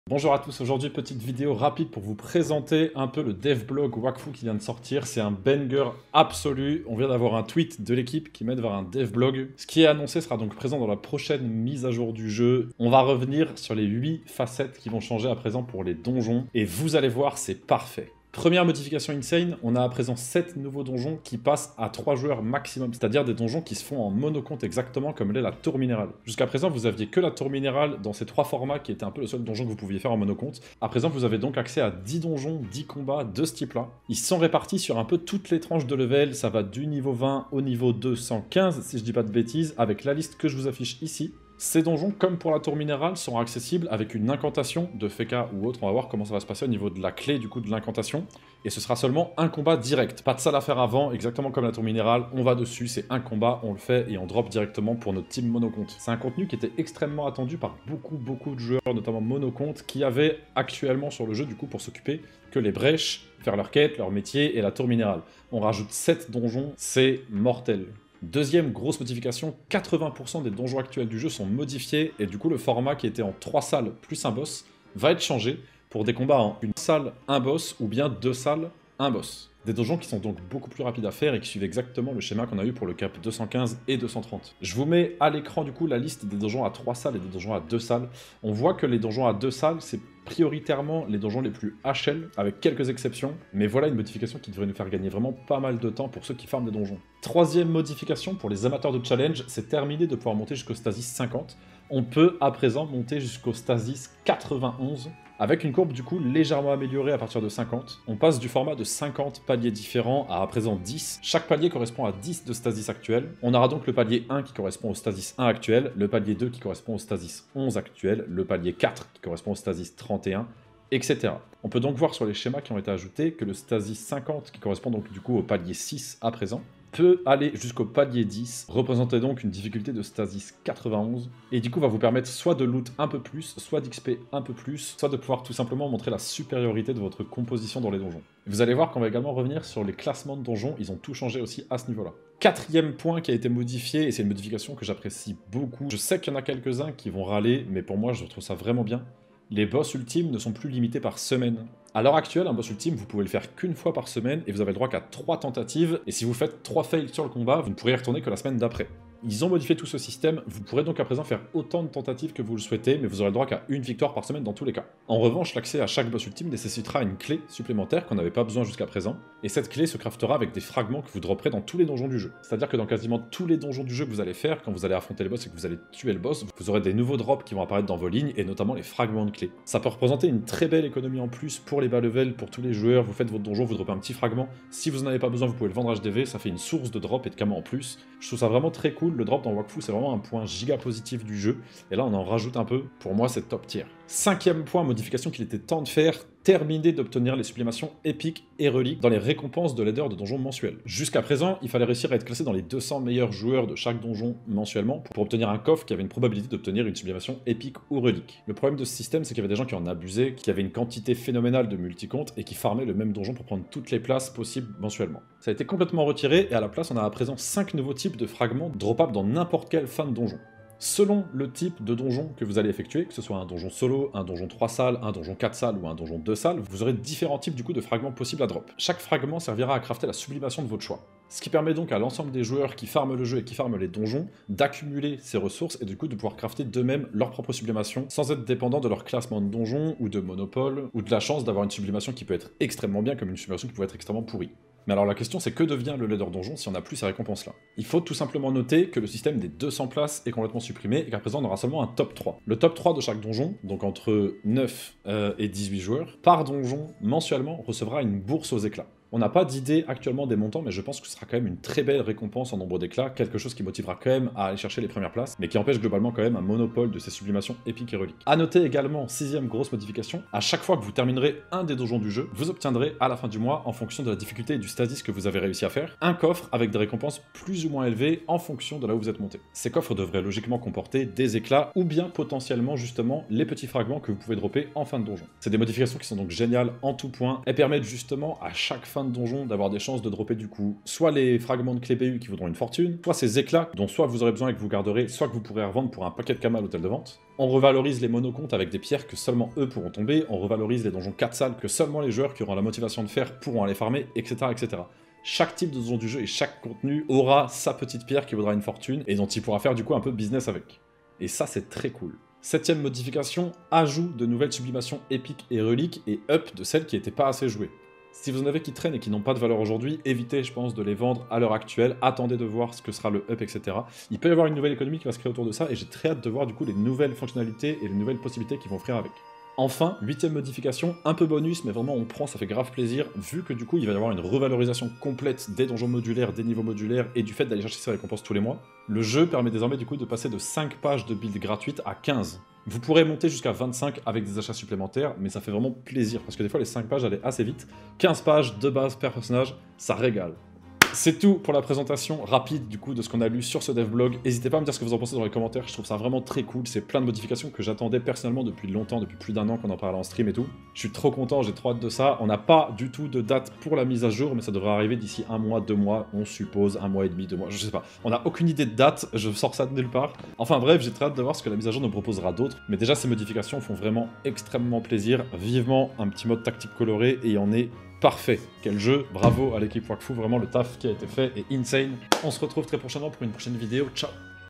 WAKFU : LA REFONTE PVM ! Donjons, Monocompte, Classements et Sublimations (ultime)
Giga banger, oubliez pas de participer au concours Instant Gaming du mois tant qu'à faire !

On analyse le devblog de la refonte des donjons de Wakfu. Que des dingueries pour les monocomptes et pour lutter contre tous les abus en tous genres.

On va se régaler.